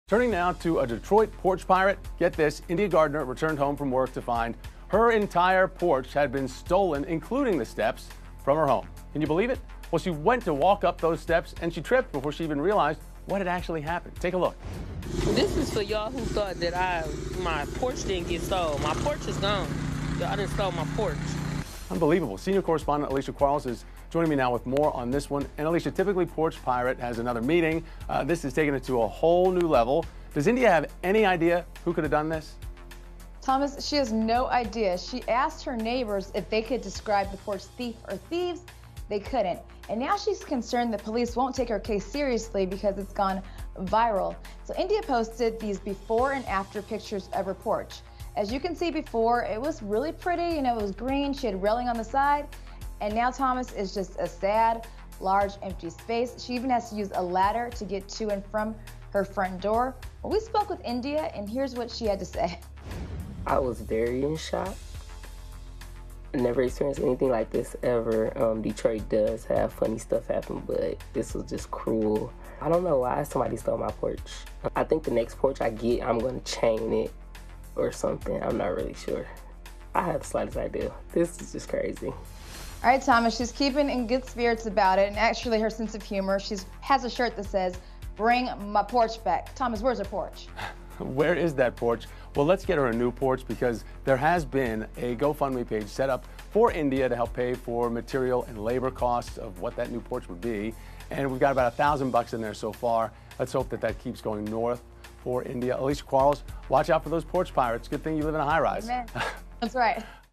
Detroit woman says thief stole her entire front porch - DailyMail TV
India Gardner from Detroit has claimed that a thief stole her entire front porch off her home while she was at work.